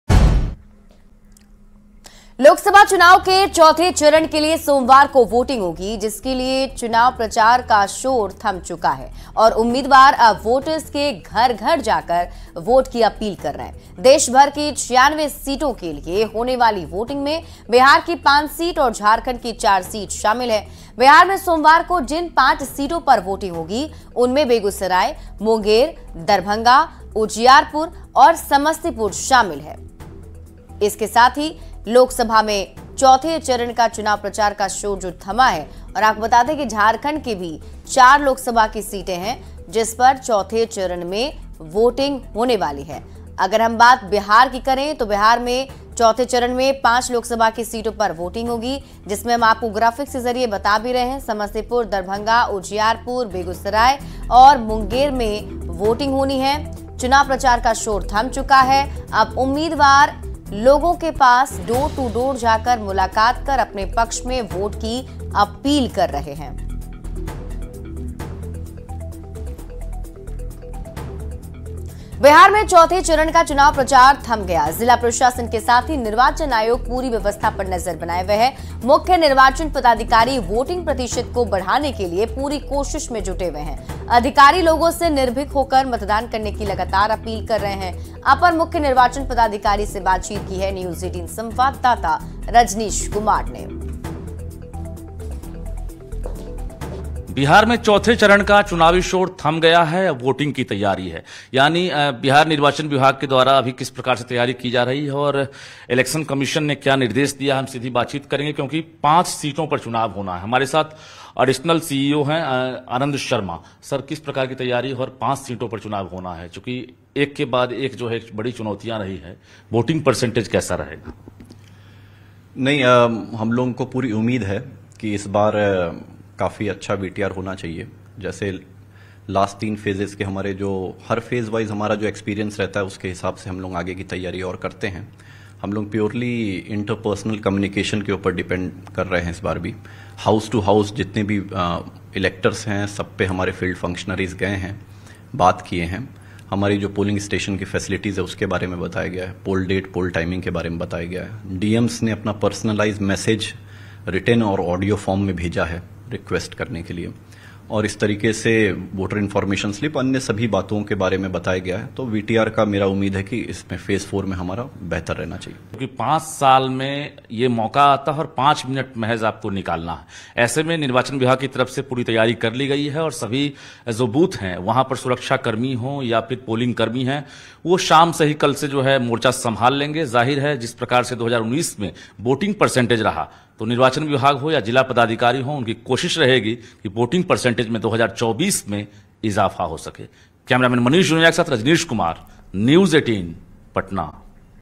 Fourth Phase Voting Bihar Jharkhand : 13 May को इन 9 सीट पर होगा मतदान | Lok Sabha Election 2024
13 मई को लोकसभा चुनाव के चौथे चरण का मतदान होगा. 19 अप्रैल से अब तक तीन चरणों में 285 निर्वाचन क्षेत्रों में मतदान हो चुका है. चौथे चरण में 10 राज्यों और केंद्र शासित प्रदेशों (UT) की 96 सीटों पर मतदान होना है.